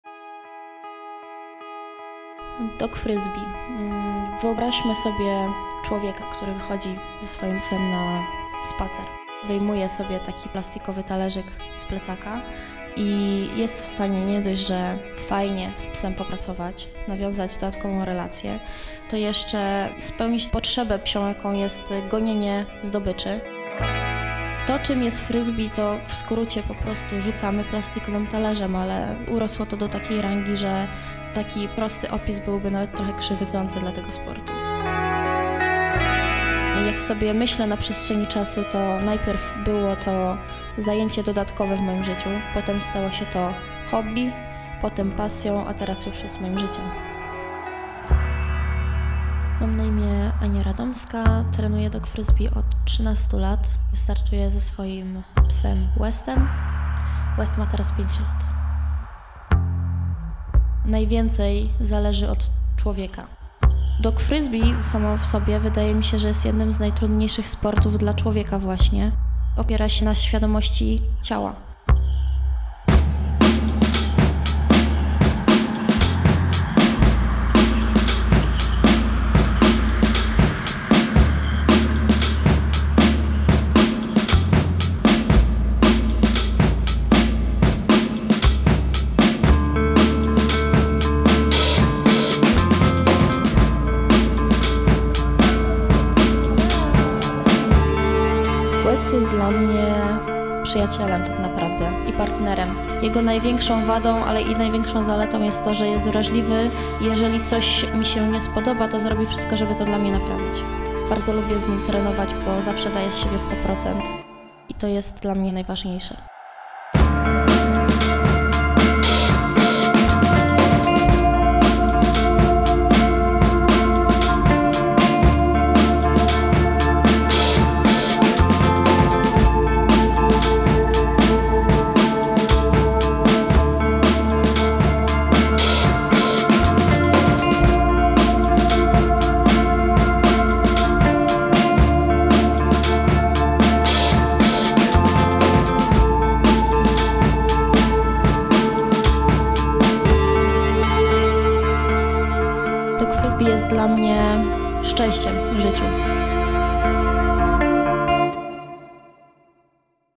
Ania Radomska&West - DOGFRISBEE BORDER COLLIE
Ania Radomska, utytułowana zawodniczka i trener Dogfrisbee, oraz jej pies border collie West w krótkiej opowieści na temat Dogfrisbee.

Ania Radomska, a titled contestant and her Border Collie West in a short story about a Dogfrisbee sport.

hauever.pl